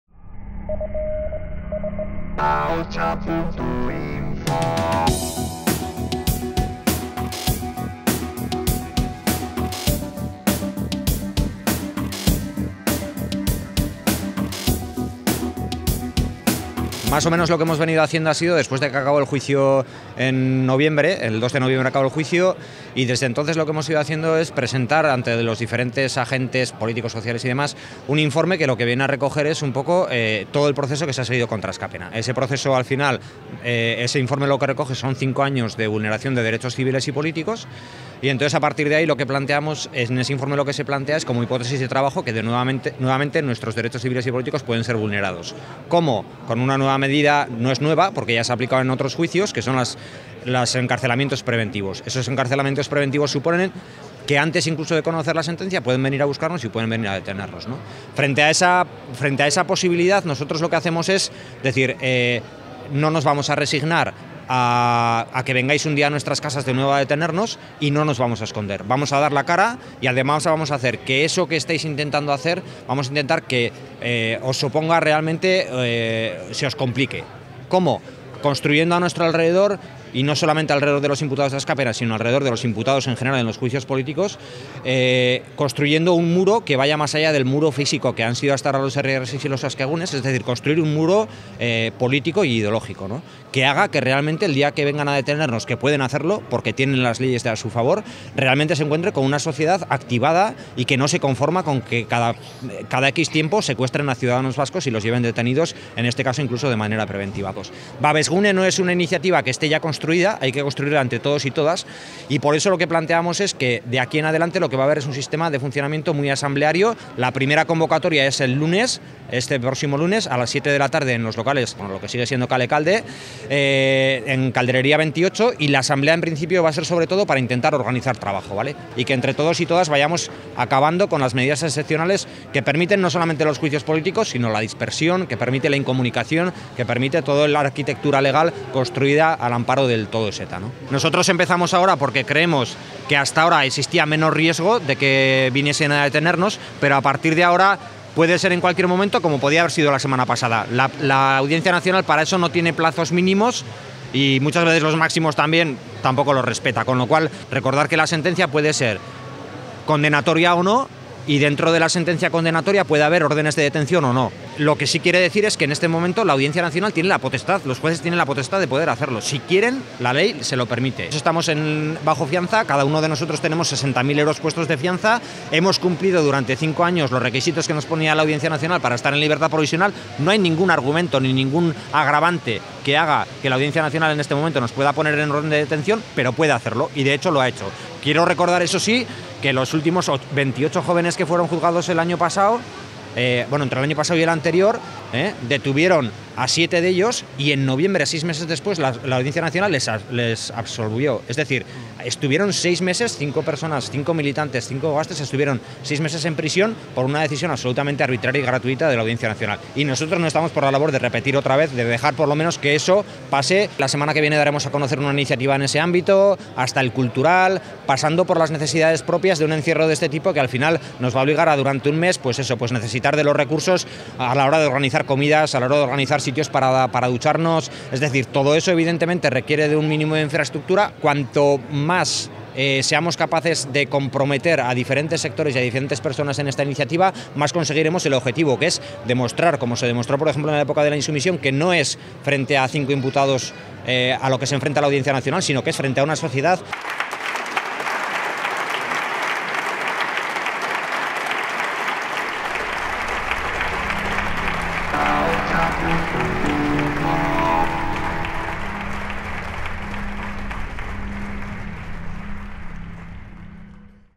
Dabid Soto: "No es a 5 imputados a quienes condena la AN, es a toda la sociedad."
Los locales de Kalekalde (Calderería, 28) de Iruñea acogen a partir de hoy el «babesgune» organizado por Askapena para denunciar el riesgo de ilicitación que sufre la organización internacionalista y la amenaza de seis años de cárcel para Walter Wendelin, David Soto, Gabi Basañez, Unai Vázquez y Aritz Ganboa.

Invierte en el cambio. Hazte accionista de AHOTSA